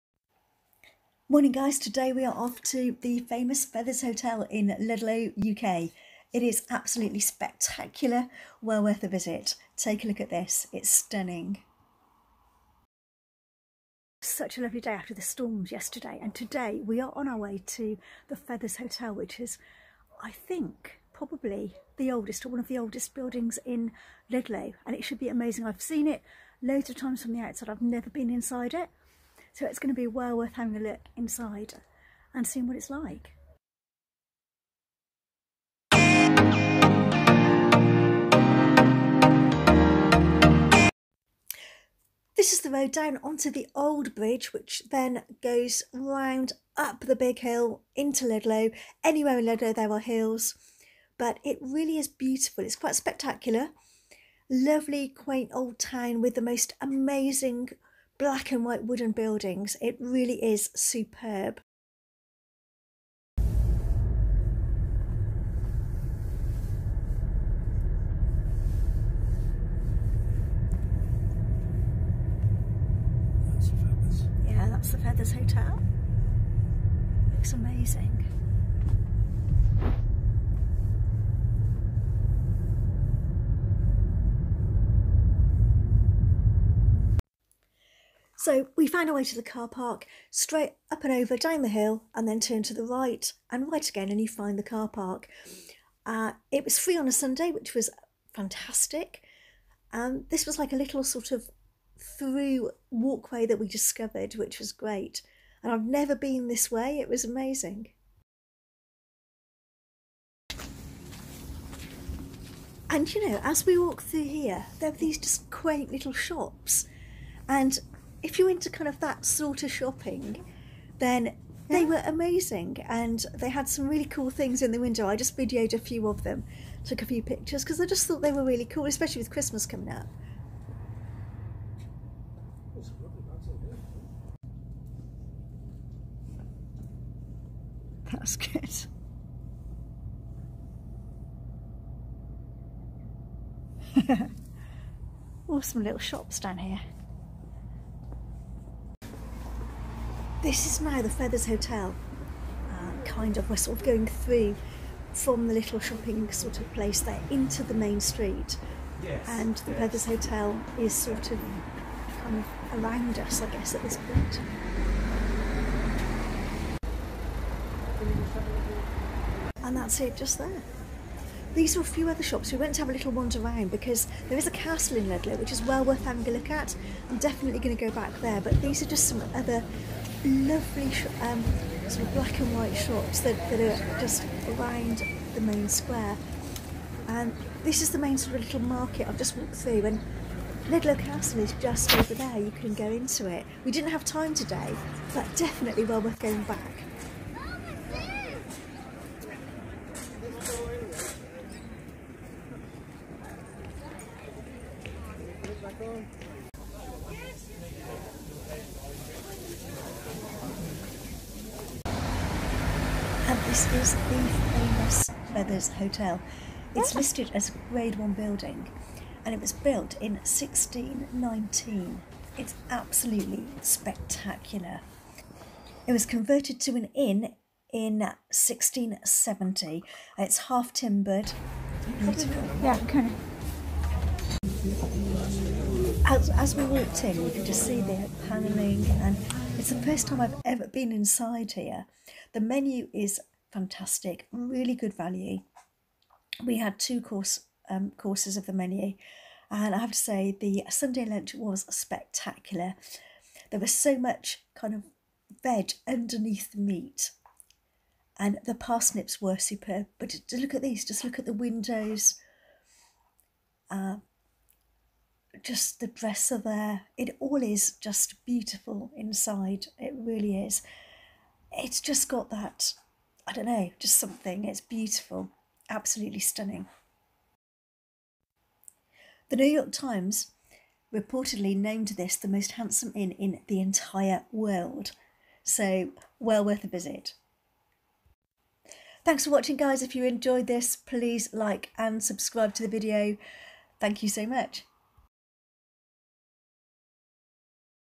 The Famous Feathers Hotel in Ludlow UK Vlog
This is the world famous Feathers hotel in the stunning quaint market town of Ludlow UK.